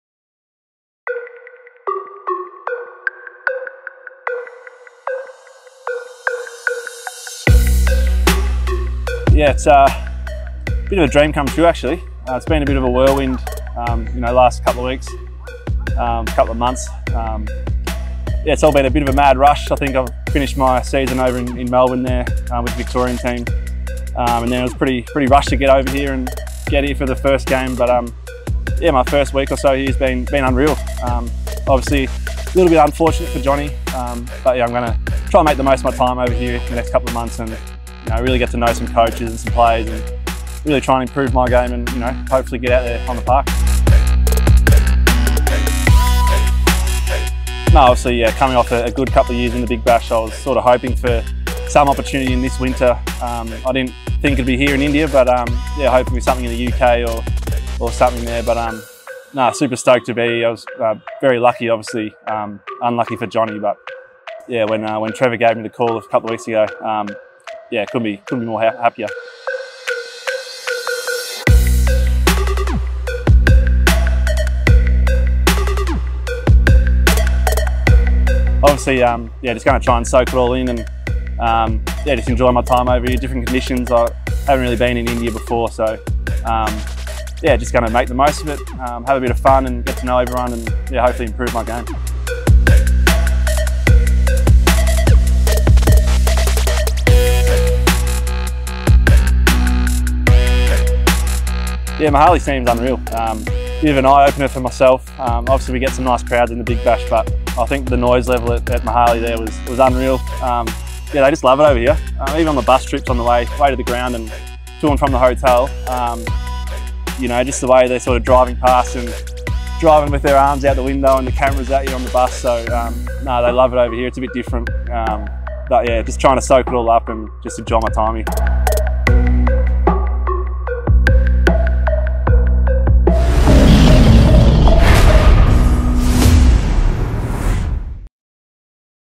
Know Sadda Sher Matt Short
Sadda Matt Short is stoked about his first-ever TATAIPL season. 🙌🏻